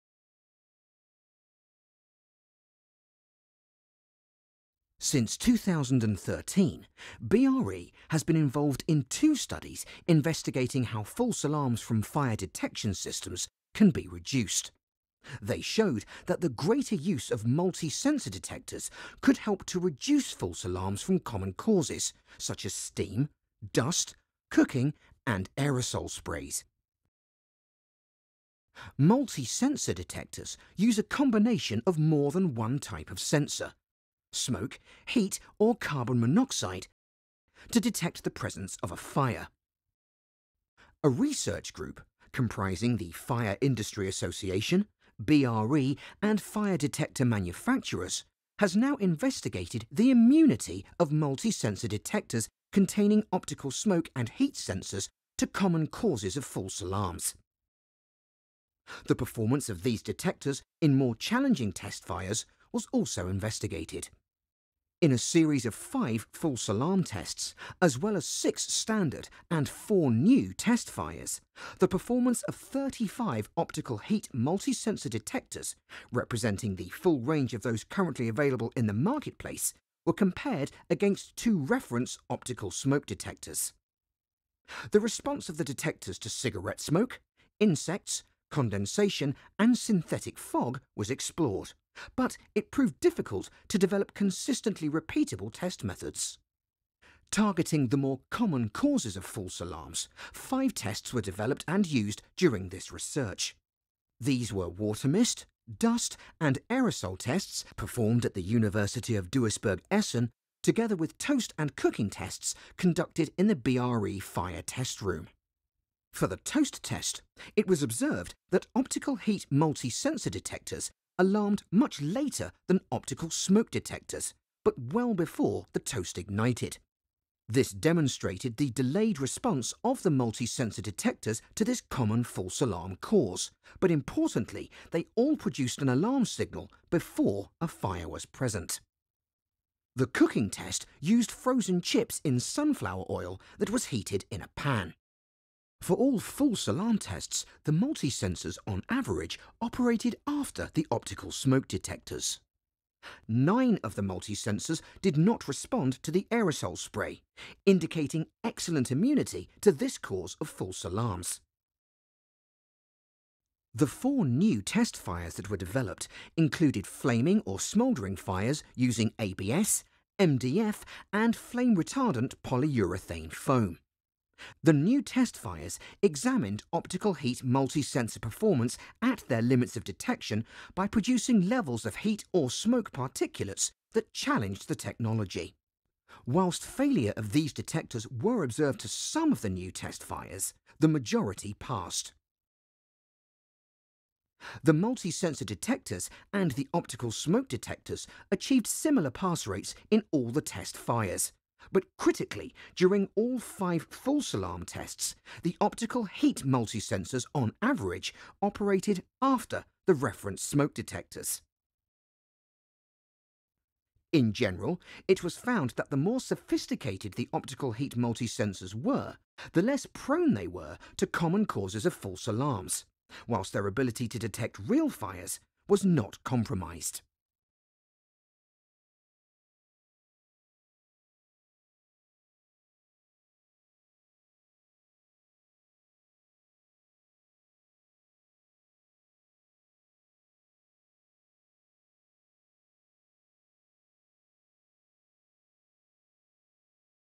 Performance of multi-sensor fire alarms
This video is part of the BRE report 'The performance of multi-sensors in fire and false alarm tests'